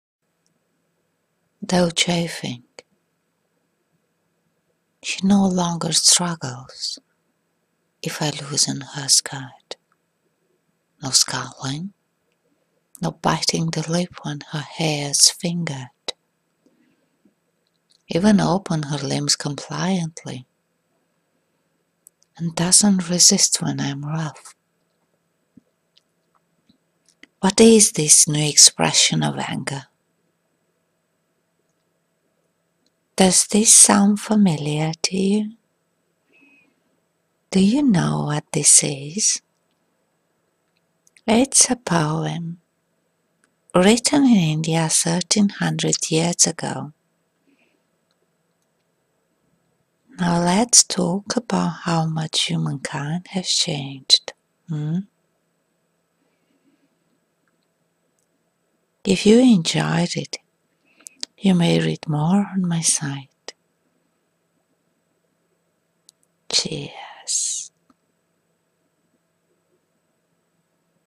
Amarushataka, Poem 63
The collection, known in Sanskrit as the Amarushataka was compiled in the eighth century and remains to this day one of the most celebrated books of poetry in India. But the provenance of this South Asian manuscripts is a complicated affair! Scholars remain divided on the most elemental details: when the Amaru collection was put together, by whom, and where.

Are you looking for a Mistress for your soul?